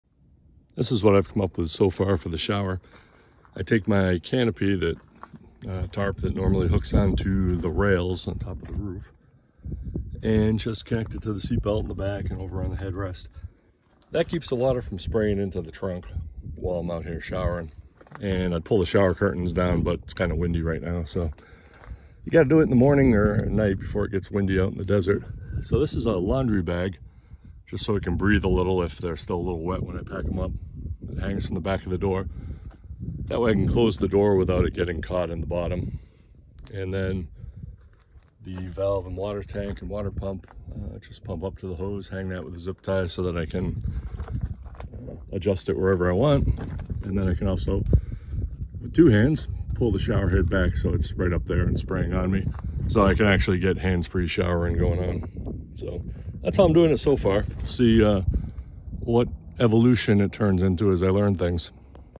A Shower in a Minivan?
Just a quick take on how I keep water out of the minivan camper, when I shower. For more details on the shower, tanks, and pump, I’ll refer you to this video:

That’s the last video I posted with an overview of the Honda Odyssey conversion that I did.